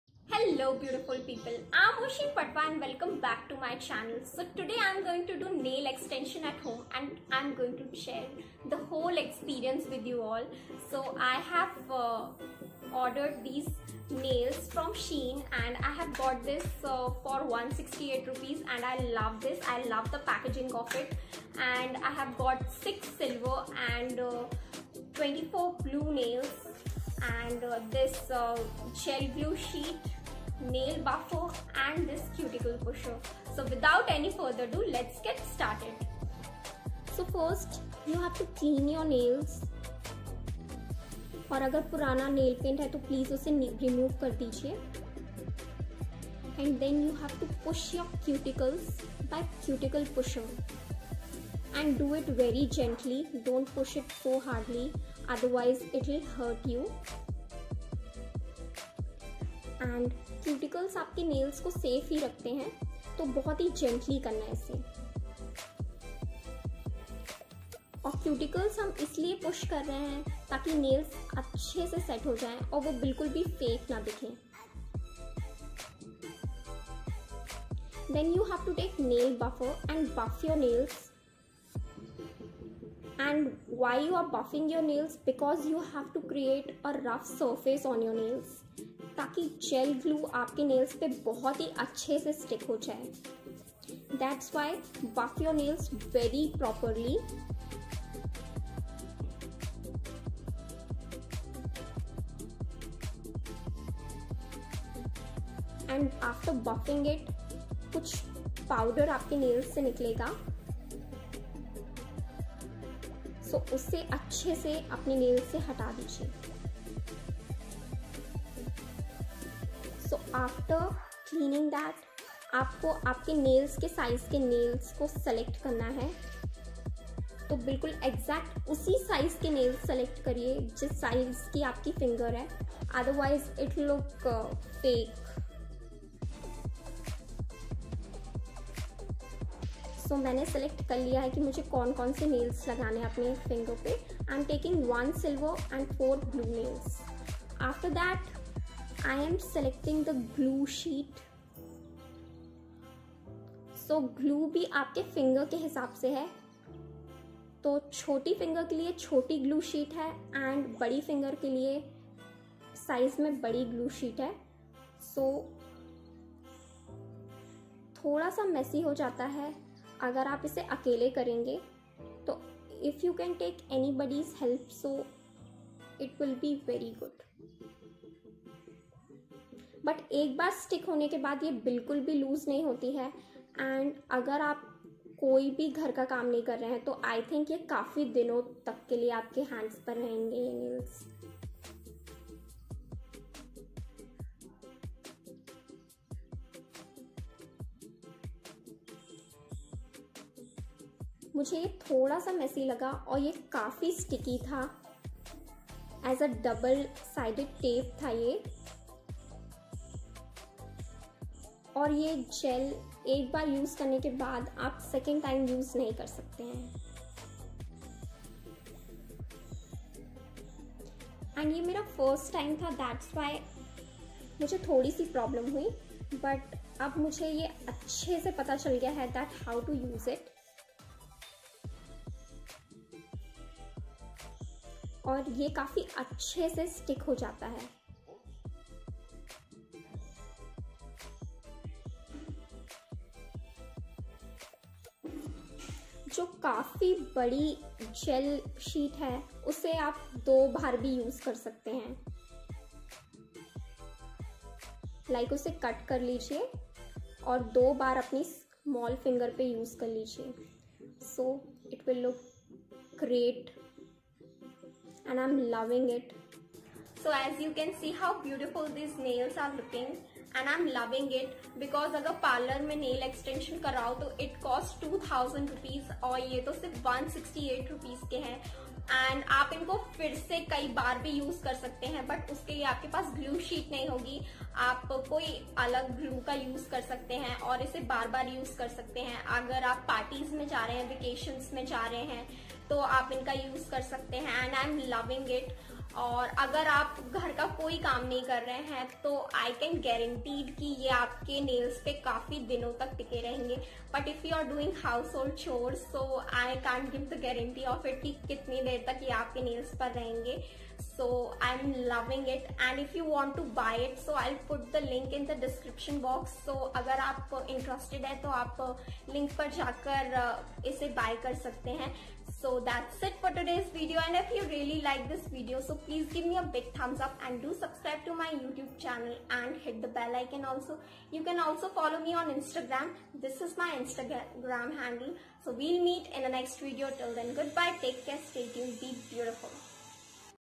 Nail Extension at Home. My First Time nail extension experience| Oshin Patwa
Hey guys I did this nail extension at my home by myself.

Musician: Jef